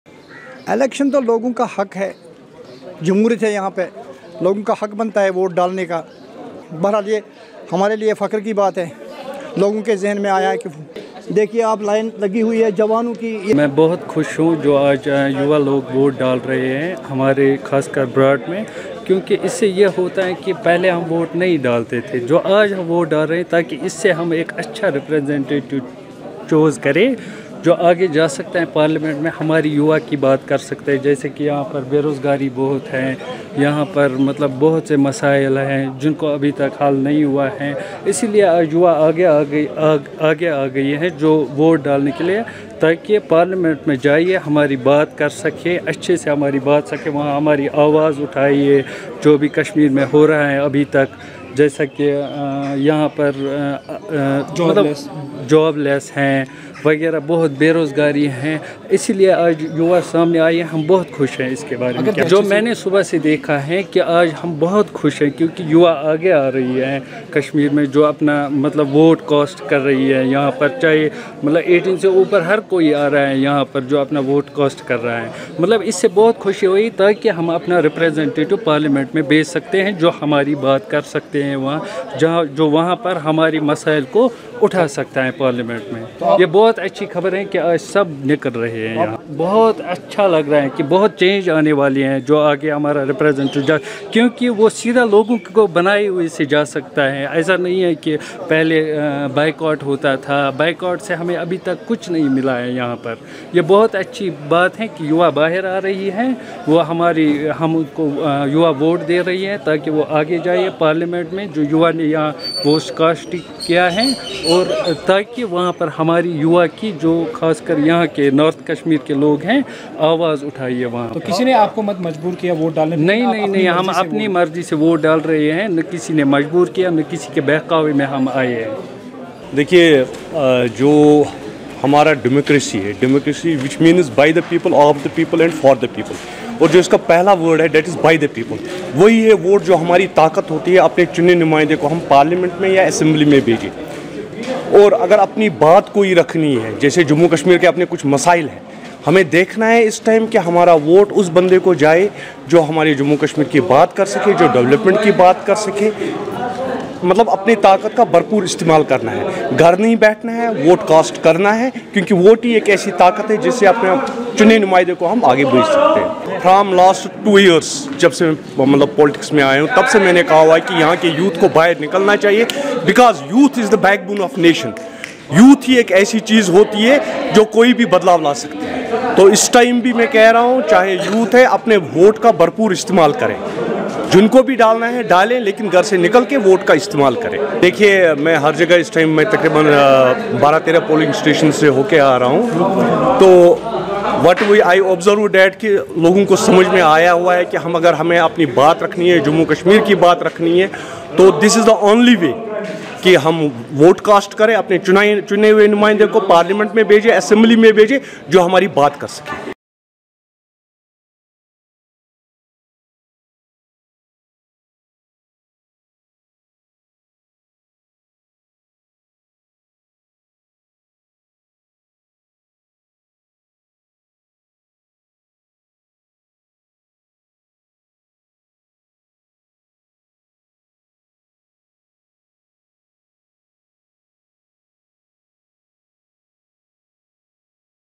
LS Polls: Voter’s Queue Up In Large Numbers In North Kashmir’s Baramulla Parliamentary Constituency
The constituency is witnessing a contest between National Conference’s Omar Abdullah, Peoples Democratic Party’s Mir Mohammad Fayaz and Jammu and Kashmir People’s Conference’s (JKPC) Sajad Lone, AIP's Er. Rashid & Others

“Voting is essential to public. With voting unemployment in Kashmir will cease to exist. Voting should be held peacefully without any disturbance…People are coming to cast their votes,” one of the voters who had come to a polling station to cast his vote said to Media.

Camera: Bilal Ahmad
Report: Aadil Bashir